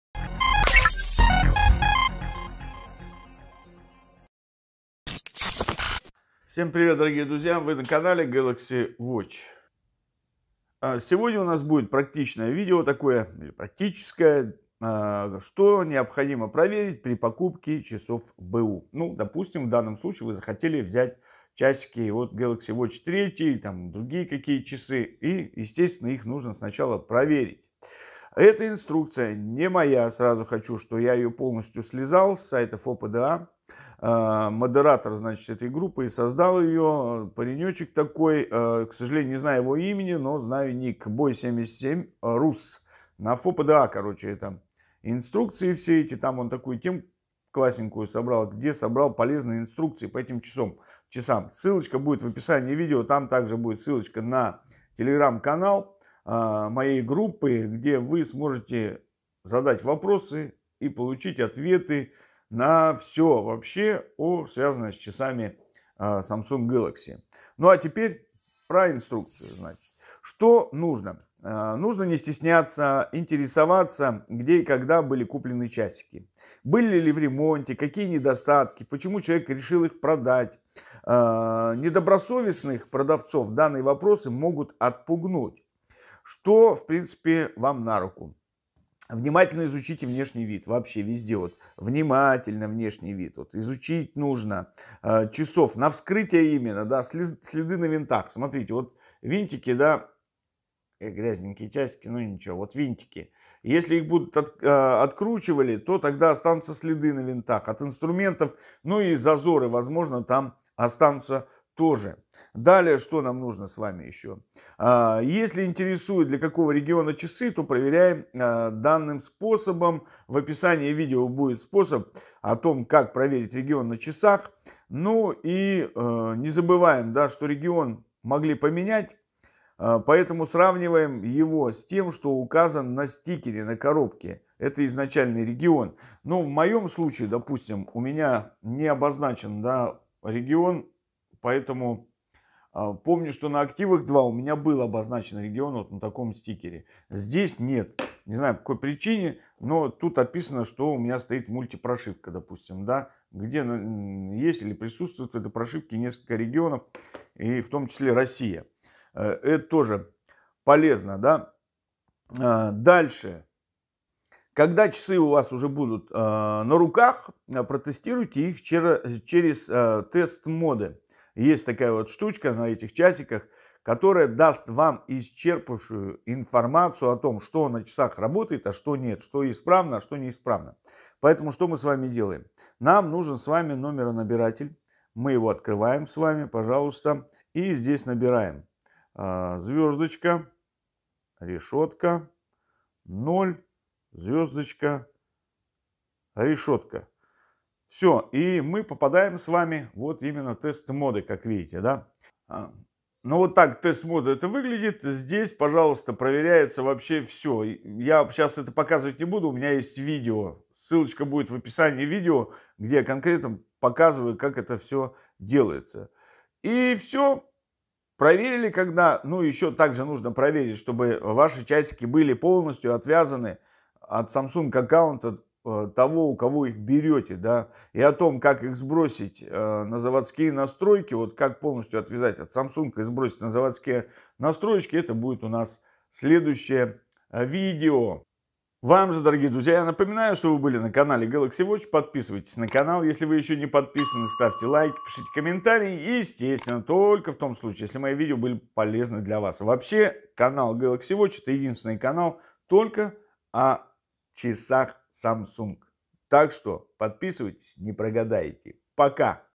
Что необходимо проверить при покупке часов Galaxy Б/У
Что необходимо проверить при покупке часов б/у?
Не стесняйтесь интересоваться, где, когда куплены часы? Были ли в ремонте, какие недостатки, почему человек решил их продавать? Недобросовестных продавцов данные вопросы могут отпугнуть, что вам на руку. Внимательно изучите внешний вид часов на вскрытие: следы на винтах, следы от инструментов, зазоры. Если интересует, для какого региона часы, то проверяем данными способами. И не забываем, что регион могли поменять, по этому сравниваем с тем, что указан на стикере на коробке (это изначальный регион). Когда часы будут у вас в руках, протестируйте их через TESTMODE.

В номеронабирателе вводим *#0*#:
Проверка цветов RGBW (на битые пиксели, например)
Vibration - Тест вибрации
Sensor:
Accelerometer - Тест акселерометра;
Barometer - Тест барометра. Результат должен быть PASS, не FALL;
Light - Тест датчика освещённости. Результат 0 на 1 и наоборот;
Gyroscope - Тест гироскопа. Результат должен быть PASS, не FALL.

Rotary Test - Тест безеля
Touch - Тест тачскрина
Sleep - Тест перевода часов в сон
Speaker - Тест динамика
Sub Key - Тест кнопок
Sensor Hub - Тест датчика сенсора, результат PASS, не FALL
HRM - Тест пульсометра
MLC - ...
Wireless Battery - Тест беспроводной зарядки
Mic Test - Тест микрофона

☕На кофе или чай:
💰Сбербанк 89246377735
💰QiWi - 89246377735